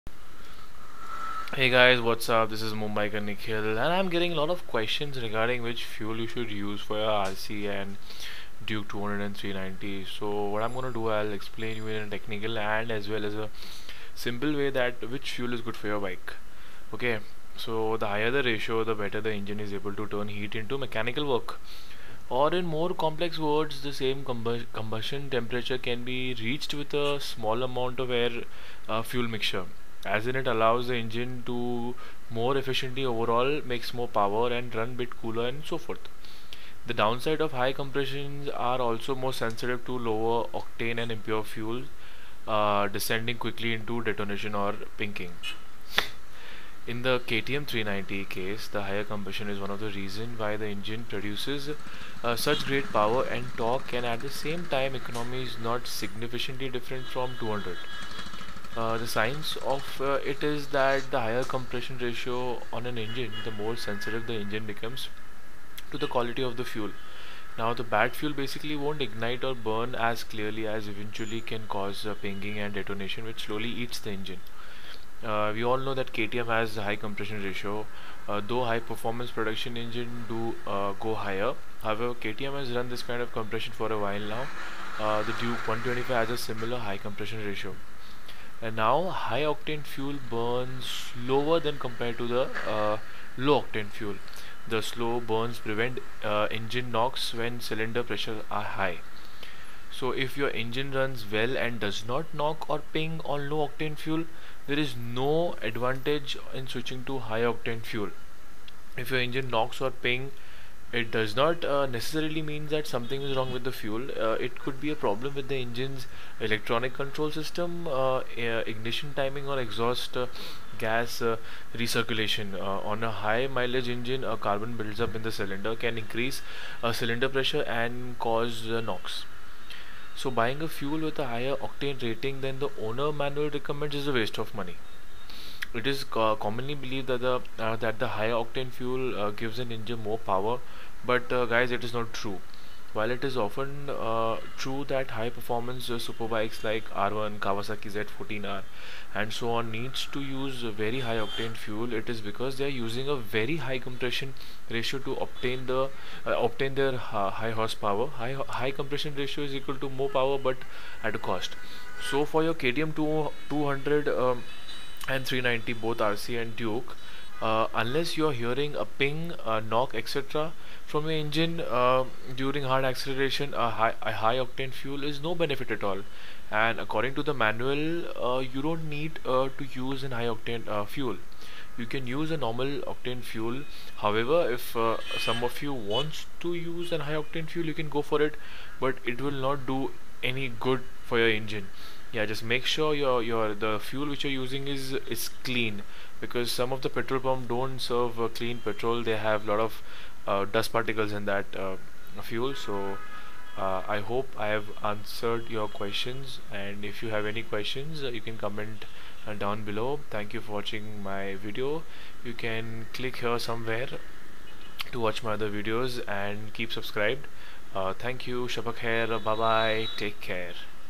KTM RC 200 and RC 390, Duke - Which Fuel to use Normal or premium ?
And i am getting lots of questions regarding which fuel you should use for RC and duke 200 and 390 so ill explain you in a technical and as well as simple way that which fuel is good for ur bike.

The higher the ratio, the better the engine is able to turn heat into mechanical work. Or in more complex words, the same combustion temperature can be reached with a smaller amount of air-fuel mixture. As in it allows the engine to more efficiently overall – make more power, run a bit cooler and so forth. The downside of a high compression are also more sensitive to lower octane and impure fuels, descending quickly into detonation or pinking.

In the KTM 390  case, the higher compression is one of the reasons why the engine produces such great power and torque and at the same time economy is not significantly different from the 200

The science of it is that the higher the compression ratio on an engine, the more sensitive the engine becomes to the quality of the fuel. Bad fuel basically won’t ignite or burn as cleanly and eventually can cause pinging or detonation which slowly eats engines. 12.6:1 is considered a high ratio, though high performance production engines do go higher. However, KTM has run this kind of compression for a while now — the Duke 125 has a similarly high compression ratio — on Indian made platforms and it seems to work, at least in the markets the Duke 125 goes to.

High octane fuel burns slower than low octane fuel. The slow burn prevents engine knock when
cylinder pressures are high.

• If your engine runs well and does not knock or ping on low octane fuel, there is no advantage in
switching to higher octane fuel.

• If your engine knocks or pings, it does not necessarily mean something is wrong with the fuel. It could
be a problem with the engine’s electronic control systems, ignition timing or exhaust gas recirculation. On a
high mileage engine, a carbon build-up in the cylinders can increase cylinder pressures and cause knock

Buying a fuel with a higher octane rating than the owners manual recommends is a waste of money.

It is commonly believed that higher octane fuels give an engine more power but this is not true.

While it is often true that high performance Superbiker like R1, kawasaki z14r and so on need to use very high octane fuel it is because they are using very high compression ratios to obtain their high horsepower. High compression ratios = more power but at a cost.

So for you KTM 200 and 390 ,

Unless you are hearing a ping (pink, knock etc) from your bikes engine during hard acceleration, a higher octane fuel will have no benefit at all.